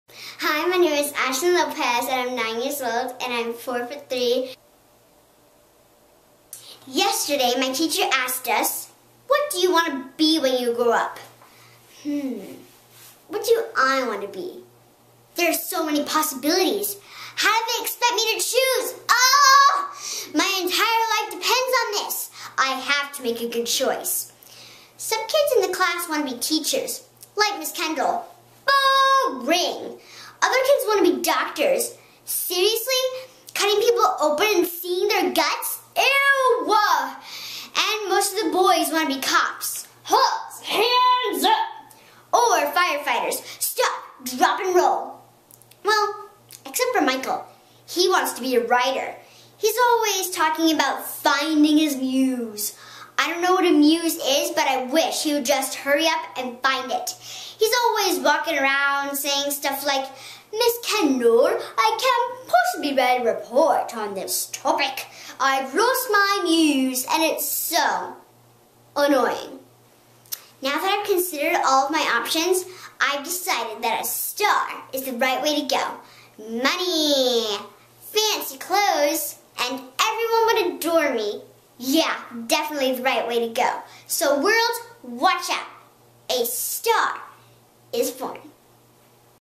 Ashlyn Jade Lopez comedy monologue
Ashlyn is a 9 yr old looking to make it on the big screen. Here is one of her comedy monolgue